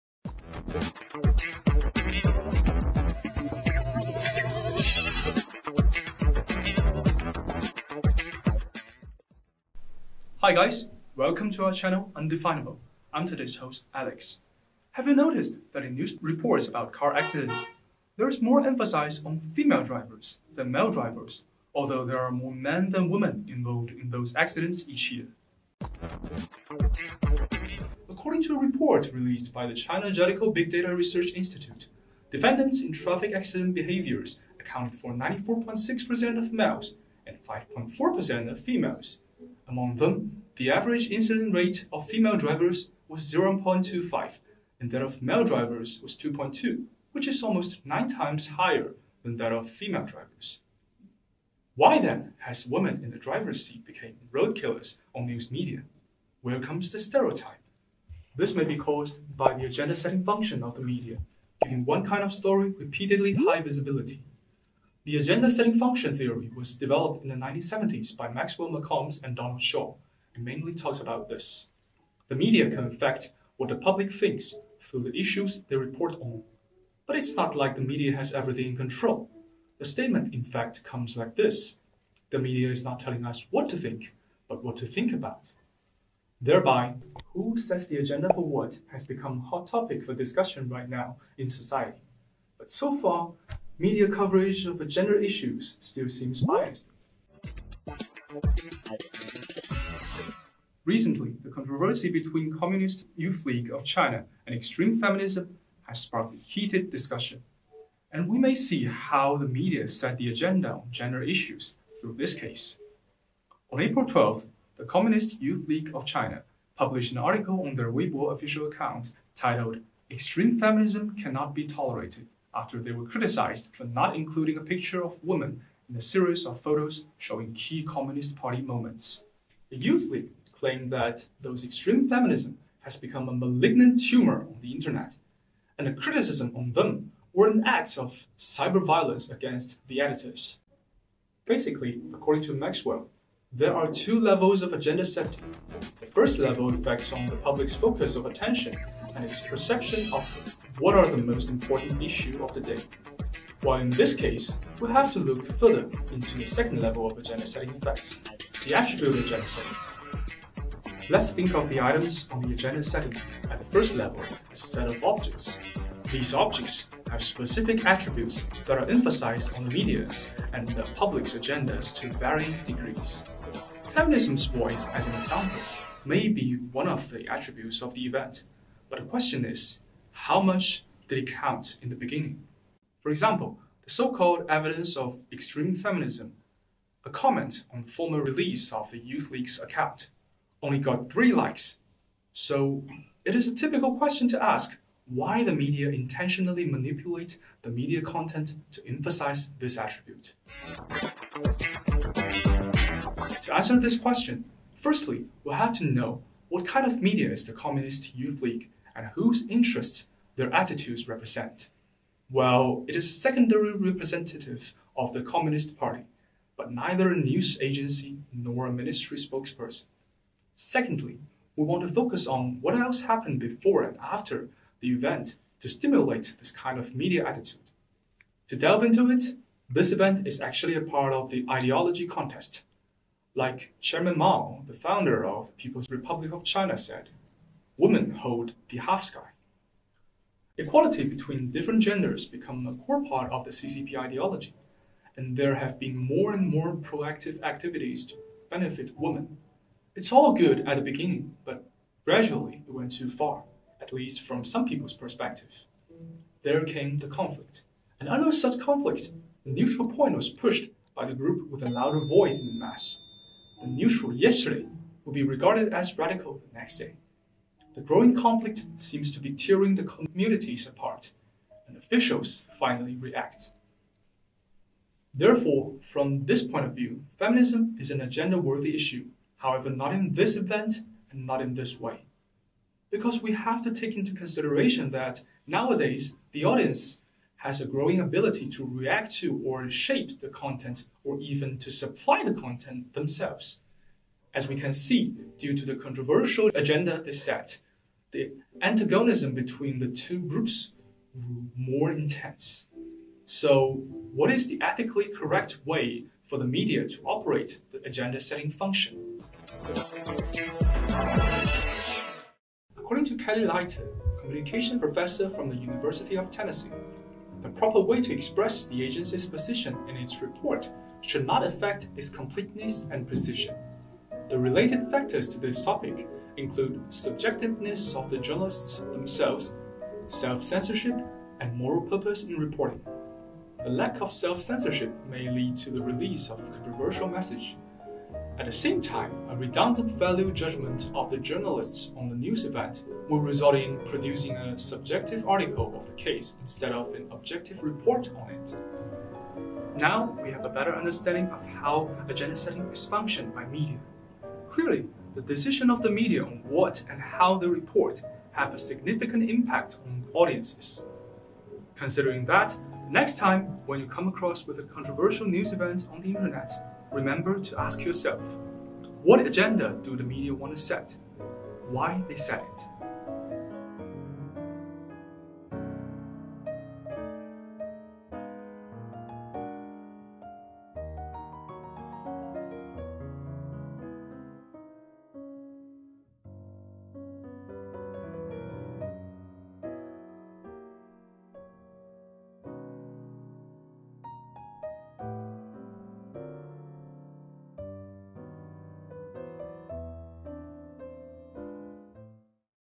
Media Literacy Education Center - The Gender Agenda
Video essay examining the interplay between agenda setting theory and gender.

Host: Luo Feicheng
Script: Feicheng and Zhong Peilin
Filmed by Liu Xiyu, Wu Yalei, Zhong Peilin
Editor: Zhong Peilin
Assistant Editors: Liu Xiyu, We Yalei, Xie Yiqi